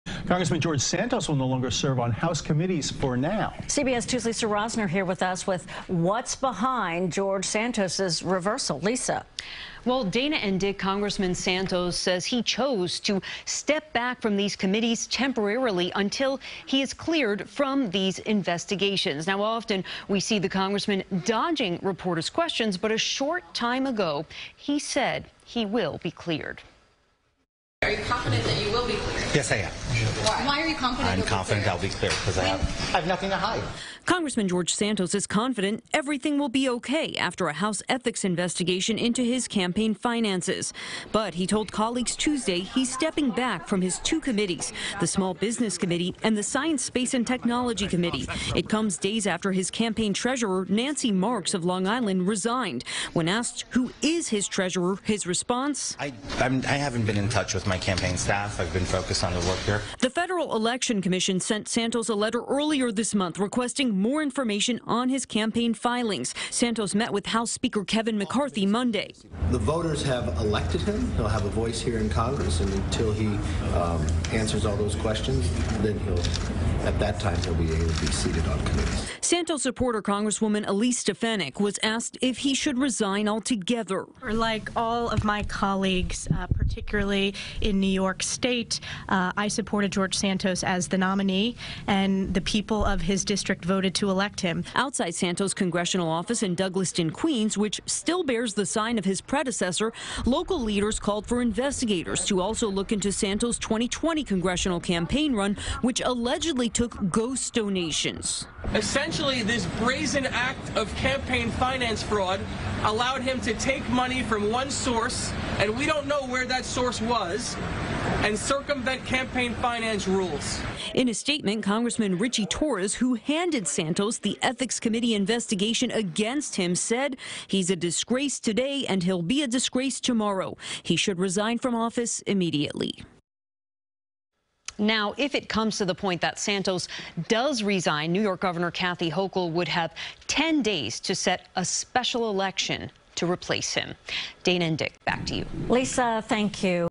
Rep. Santos says he's temporarily stepping back from House committees
Congressman George Santos will no longer serve on House committees, for now. CBS2's Lisa Rozner has more on what's behind the lawmaker's reversal.